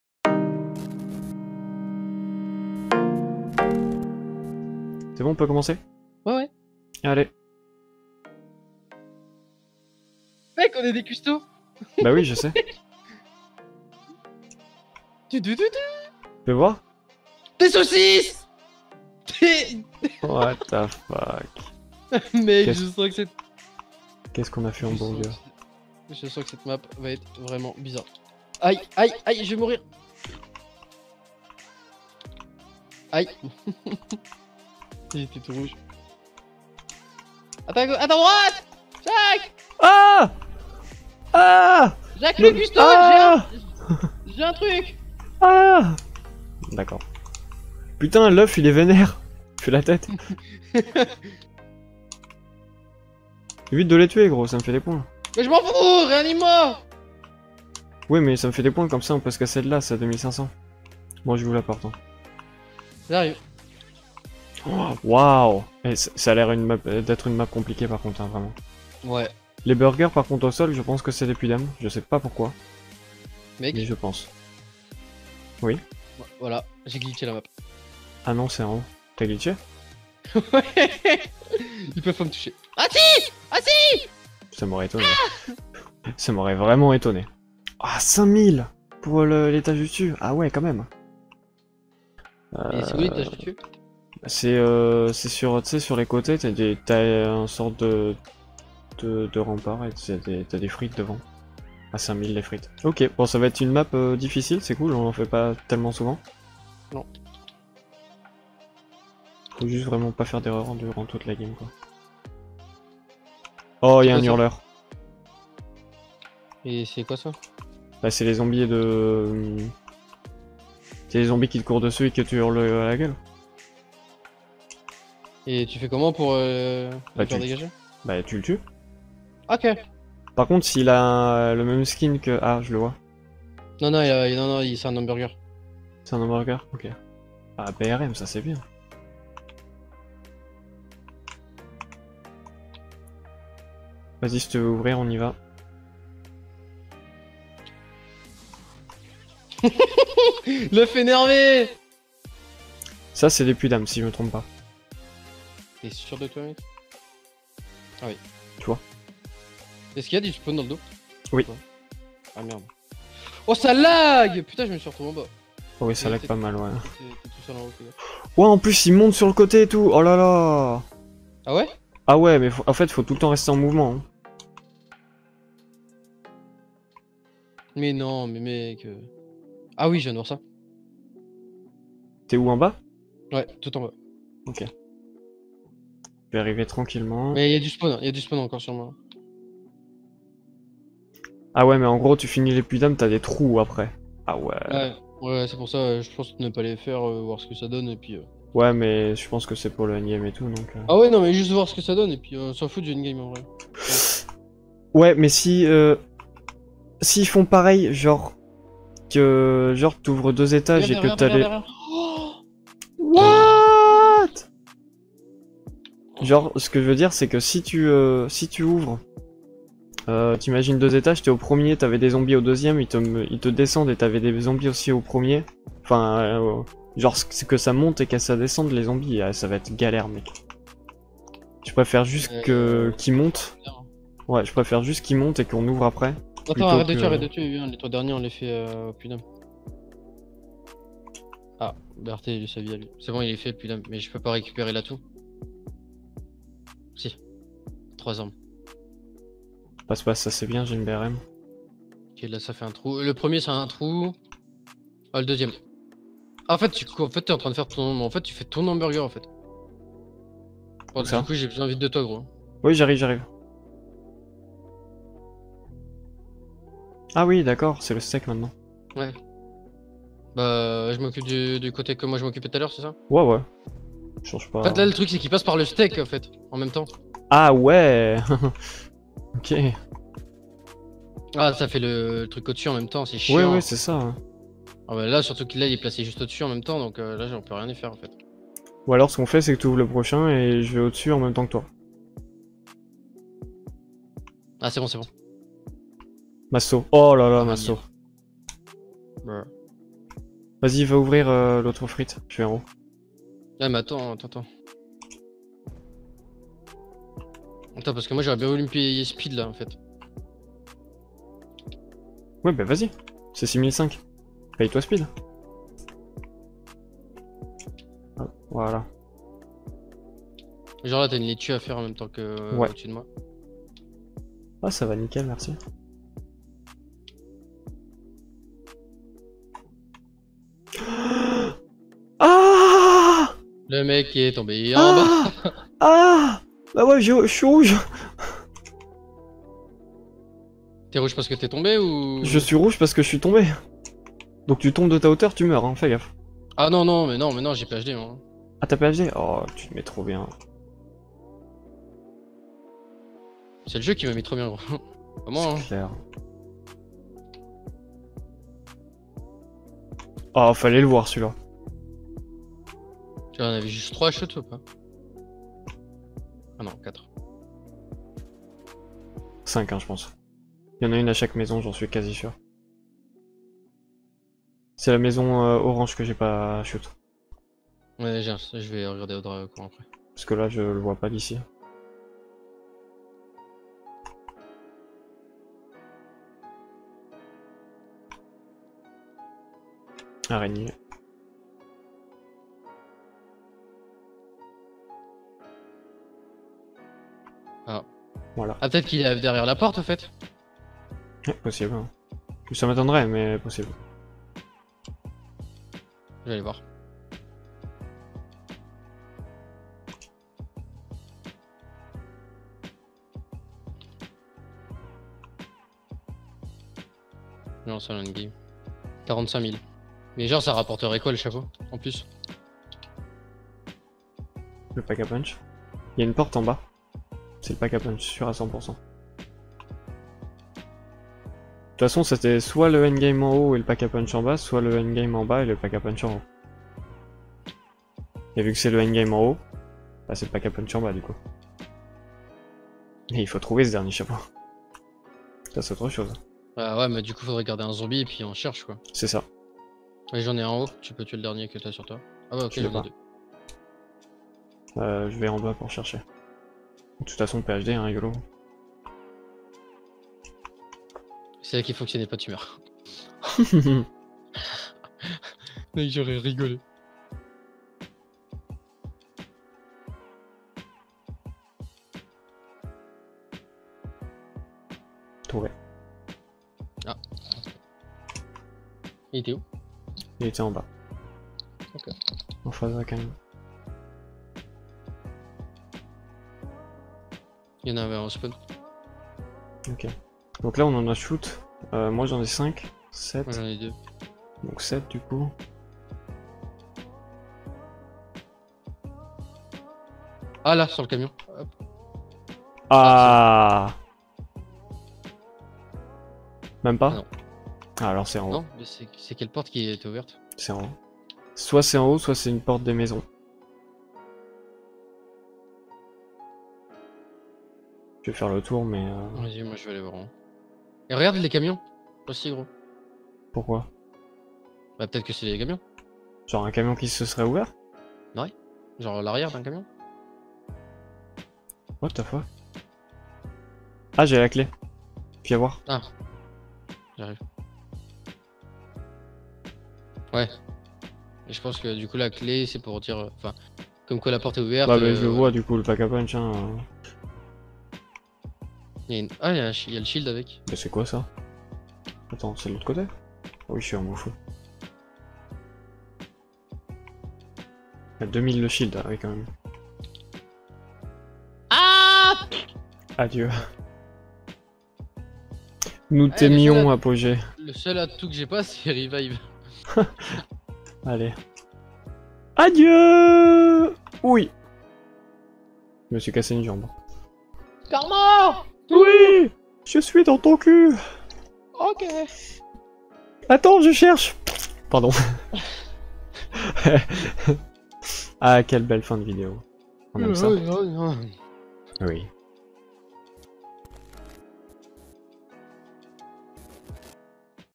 TUTO CUISINE
Comment faire un hamburger sans être gêner par des Zombies ?